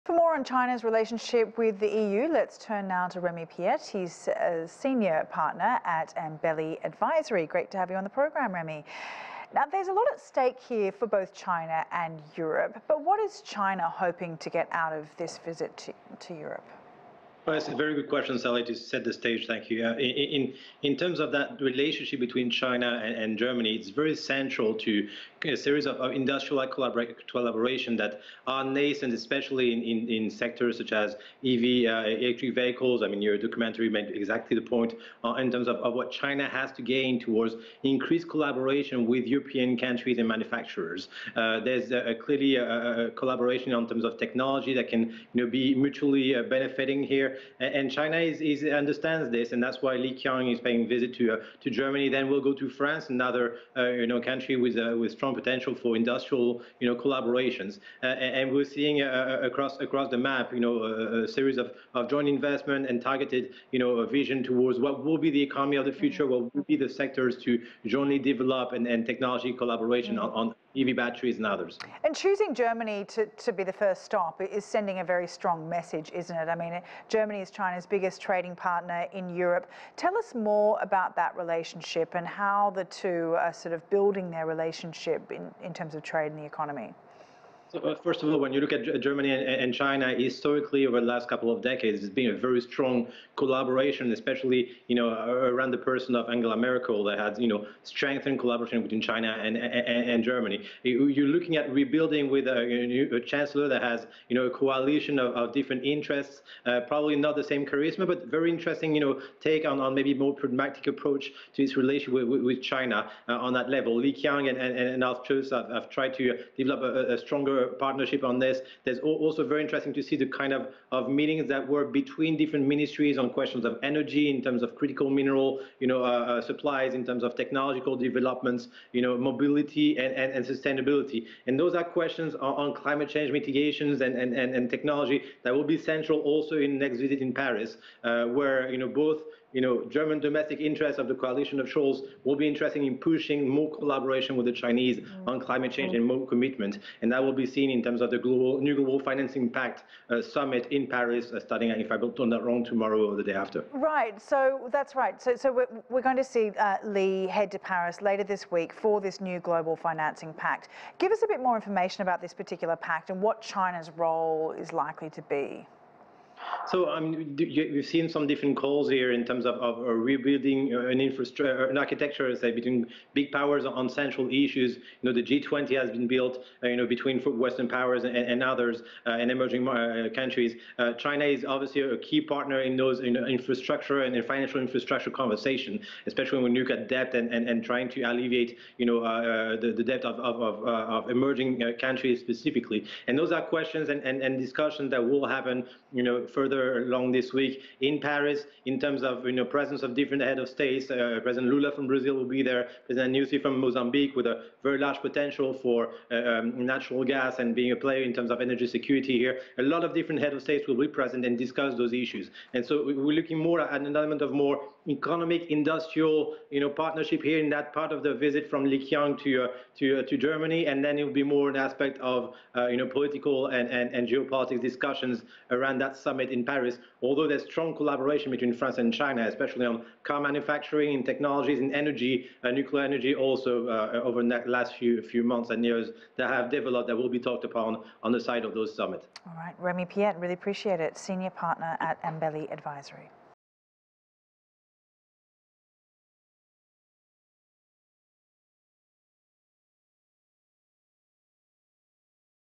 Remi Piet on Chinese Premier Li Qiang's EU Visit
CGTN's Sally Ayhan speaks with Remi Piet, Senior Partner at Embellie Advisory, about Chinese Premier Li Qiang's first trip to Europe in his new role.